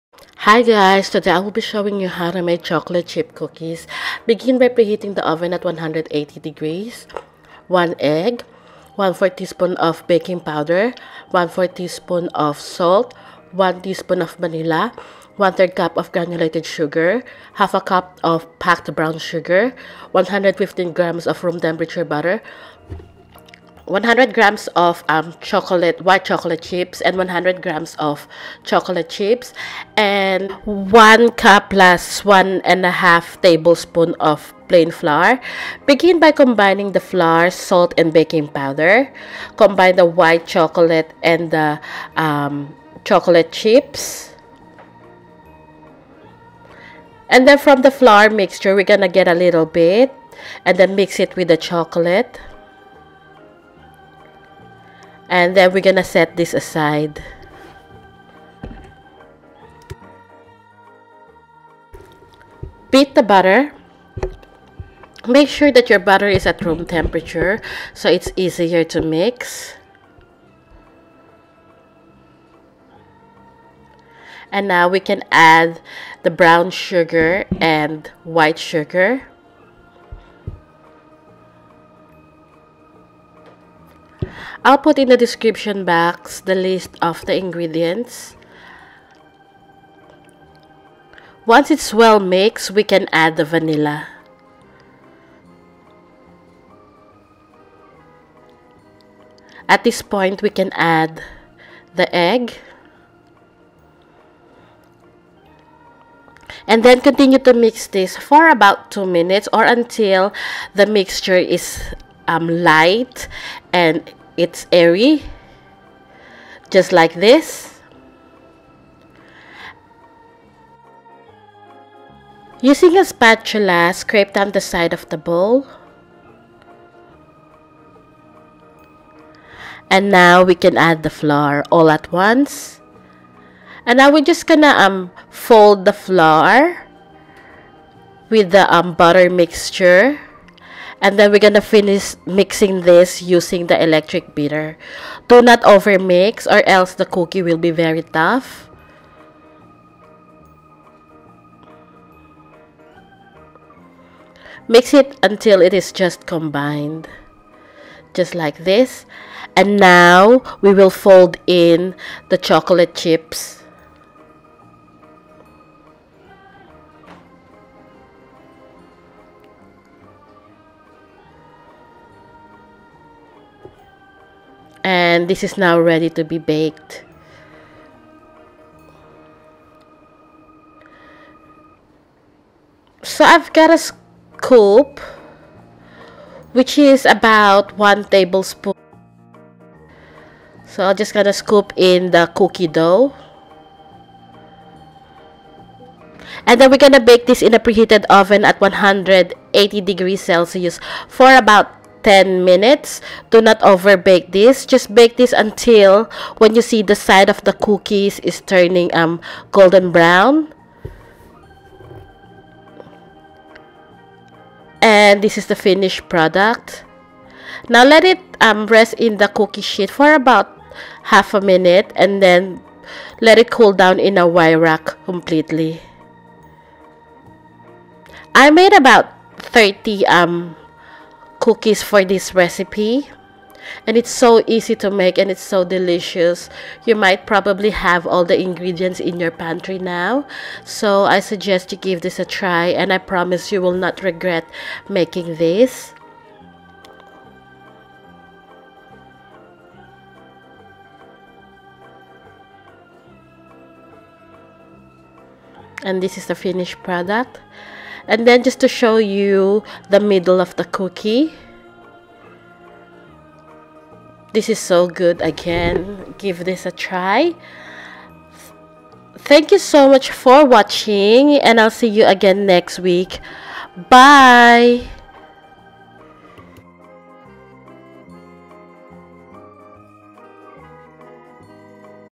Chocolate Chip Cookie
115 g butter
1 egg
1/2 cup packed brown sugar
1/3 cup granulated white sugar
1 tsp vanilla
1 cup +1 1/2 tbsp plain flour
100 g chocolate chips
100 g white chocolate chips
1/4 tsp baking powder